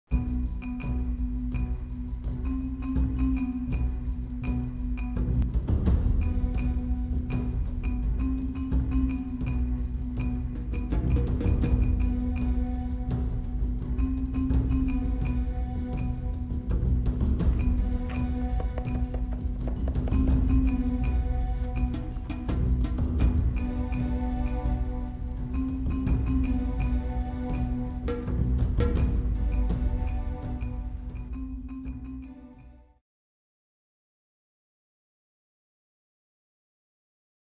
Cat vs Polar Bear - Cat kicks polar bear
The polar bear has no chance ^-^
Please watch in HD for a better quality video. Wall-e's twitter;
Recorded with Canon Ixus125HS